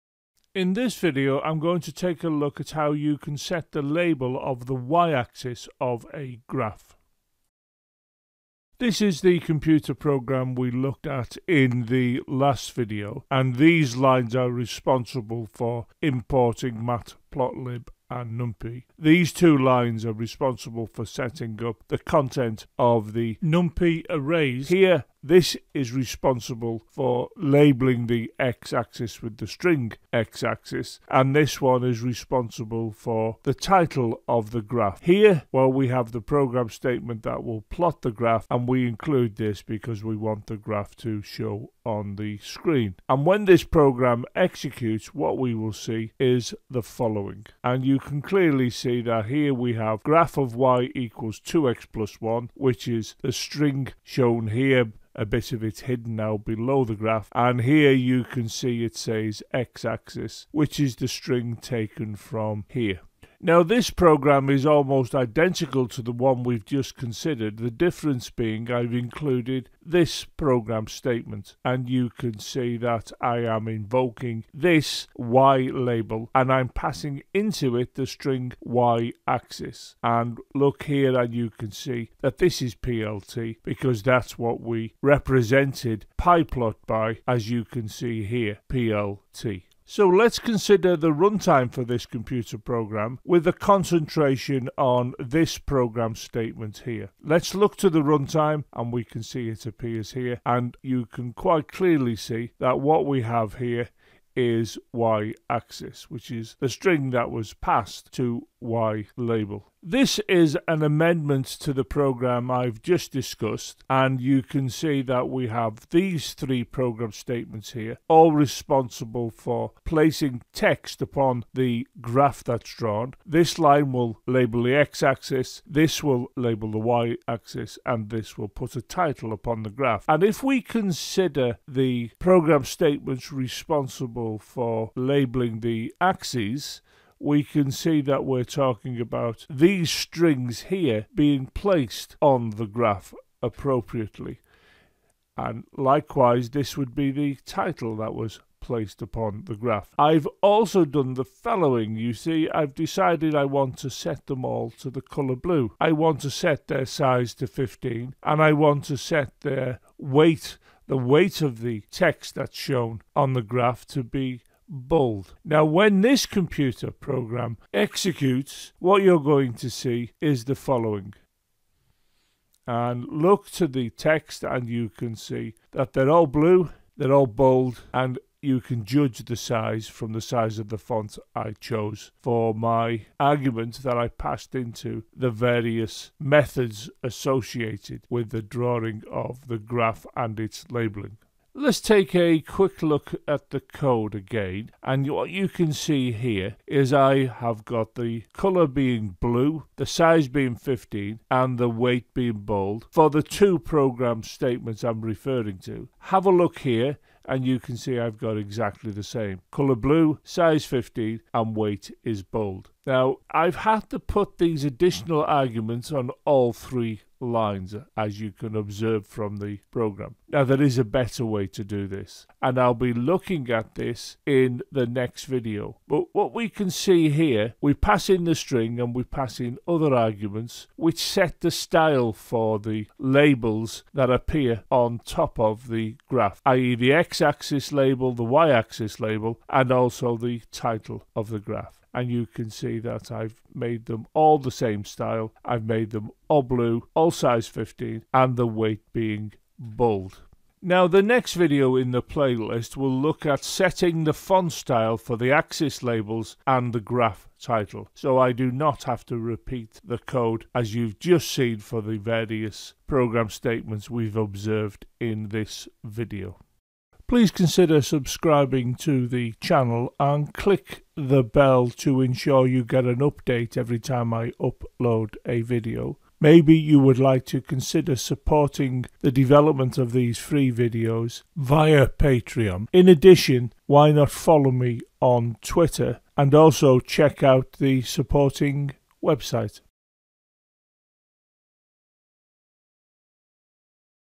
Mastering Matplotlib: How to Set the Y-Axis Label in Pyplot
Looks at how the y axis of a graph can be labelled.